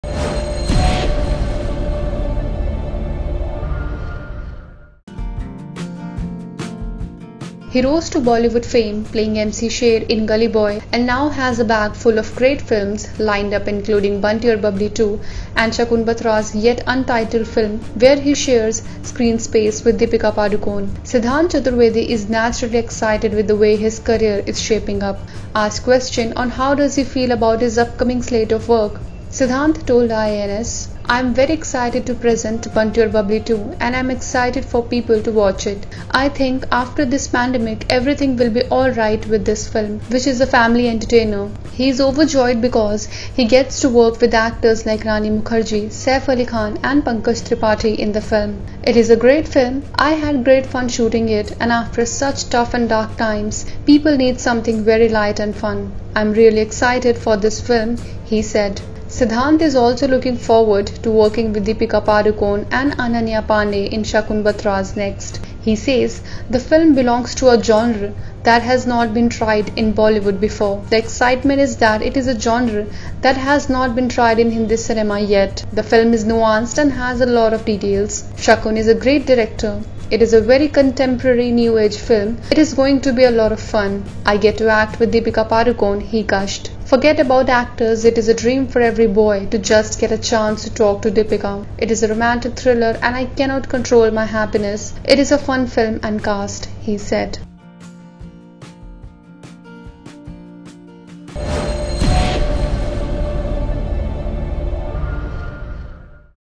Siddhant Chaturvedi gushes about working with Deepika Padukone next
He rose to Bollywood fame playing MC Sher in "Gully Boy" and now has a bag full of great films lined up including "Bunty Aur Babli 2" and Shakun Batra's yet-untitled film where he shares screen space with Deepika Padukone.